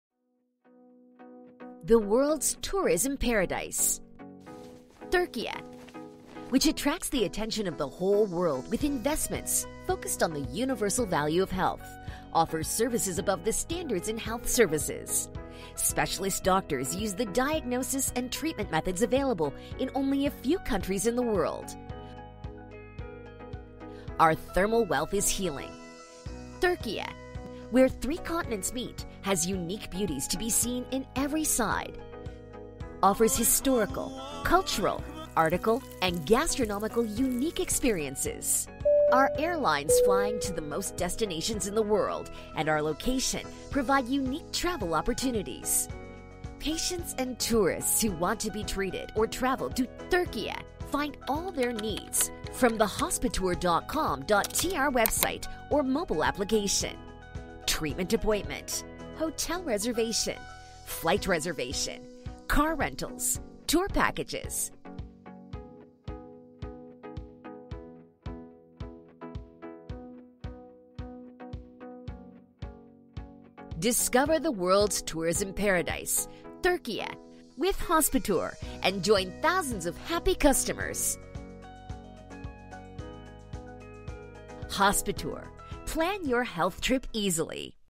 Hospitour Medical Tourism - Official Trailer
The most innovative medical tourism portal. Hospitour!

You can make Treatment Appointment, Hotel Accommodation Reservation, Flight Reservation, Car Rental Reservation and Tour Package Reservation. You can also find the nearest pharmacy to you with Hospitour.

Hospitour offers you opportunity to earn miles on all reservations you make, and you also earn miles from airline programs. By using the miles you earn in all reservations, you can easily plan your medical trip at the most advantageous prices.

Hospitour is an initiative that transforms medical tourism in the world by digitizing.

Sign up to Hospitour now and plan your health trip safely and affordably!